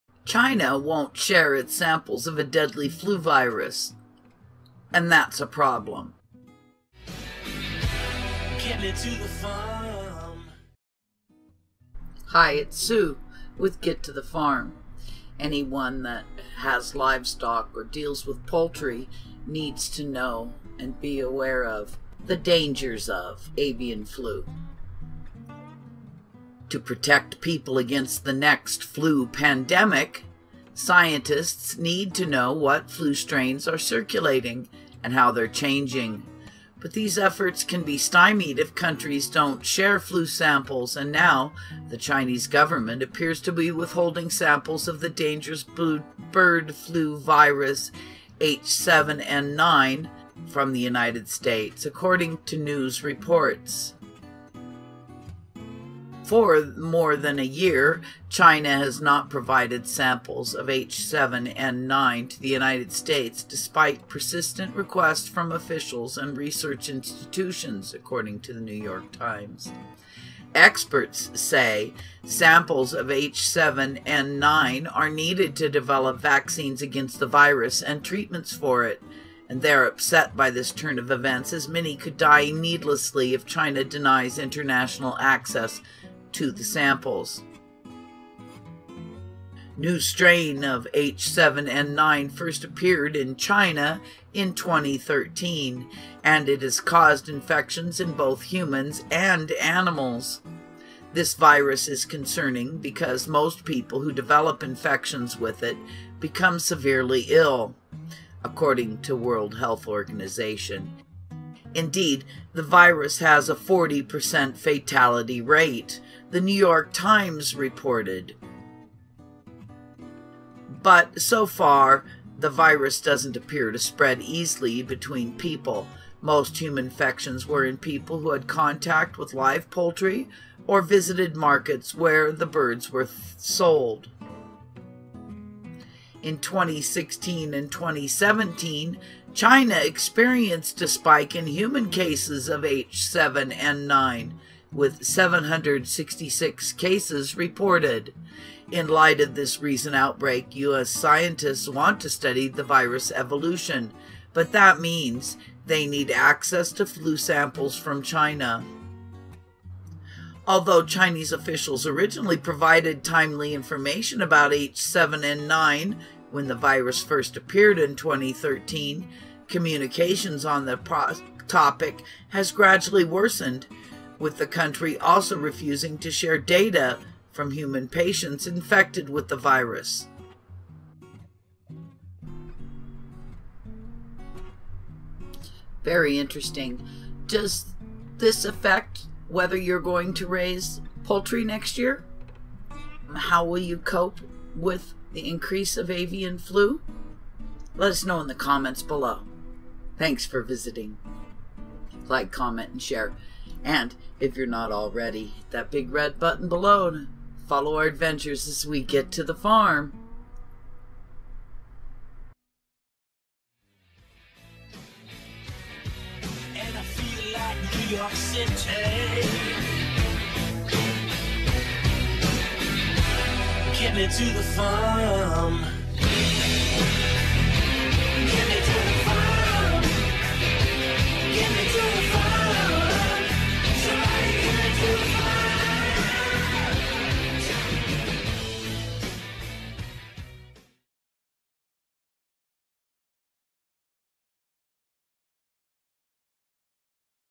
China Won't Share Its Samples of a Deadly Flu Virus!!! Trouble ahead
China Won't Share Its Samples of a Deadly Flu Virus. Here's Why That's a Problem.

To protect people against the next flu pandemic, scientists need to know what flu strains are circulating and how they are changing. But such efforts can be stymied if countries don't share flu samples, and now, the Chinese government appears to be withholding samples of the dangerous bird flu virus H7N9 from the United States, according to news reports.

For more than a year, China has not provided samples of H7N9 to the United States, despite persistent requests from officials and research institutions, according to The New York Times.

Experts say samples of H7N9 are needed to develop vaccines against the virus and treatments for it- they are upset by this turn of events as many could die needlessly if China denies international access to samples.

A new strain of H7N9 first appeared in China in 2013, and it has caused infections in both humans and animals. This virus is concerning because most people who develop infections with it become severely ill, according to the World Health Organization (WHO). Indeed, the virus has a 40-percent fatality rate, The New York Times reported. But so far, the virus doesn't appear to spread easily between people; most human infections were in people who'd had contact with live poultry or visited markets where the birds were sold.

In 2016 to 2017, China experienced a spike in human cases of H7N9, with 766 cases reported. In light of this recent outbreak, U.S. scientists want to study the virus's evolution. But this means they need access to flu samples from China.

Although Chinese officials originally provided timely information about H7N9 when the virus first appeared in 2013, communication on the topic has gradually worsened, with the country also refusing to share data from human patients infected with the virus, The New York Times reported.